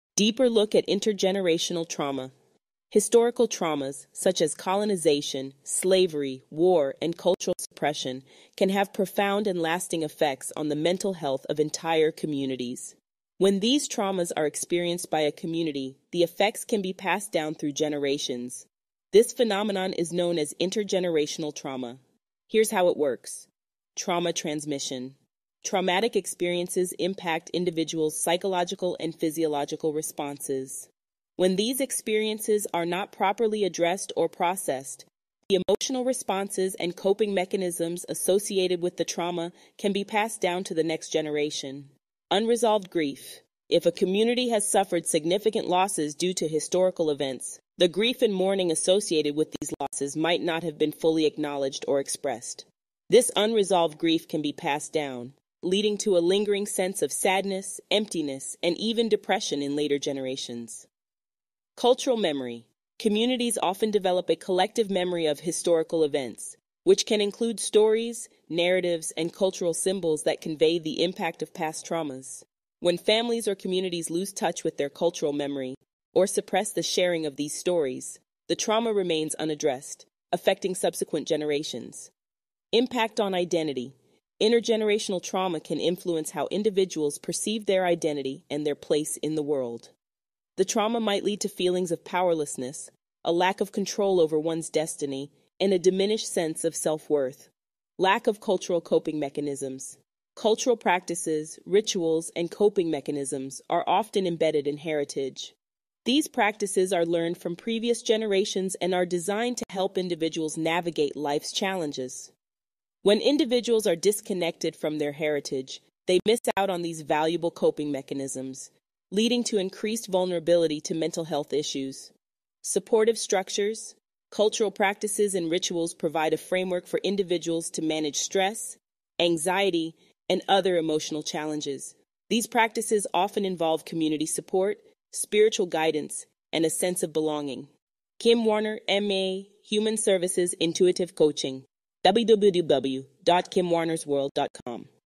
Intergenerational trauma effects
Impact on Identity: Intergenerational trauma can influence how individuals perceive their identity and their place in the world. The trauma might lead to feelings of powerlessness, a lack of control over one's destiny, and a diminished sense of self-worth. Lack of Cultural Coping Mechanisms: Cultural practices, rituals, and coping mechanisms are often embedded in heritage. These practices are learned from previous generations and are designed to help individuals navigate life's challenges. When individuals are disconnected from their heritage, they miss out on these valuable coping mechanisms, leading to increased vulnerability to mental health issues: Supportive Structures: Cultural practices and rituals provide a framework for individuals to manage stress, anxiety, and other emotional challenges. These practices often involve community support, spiritual guidance, and a sense of belonging. Kim Warner M.A Human Services Intuitive coaching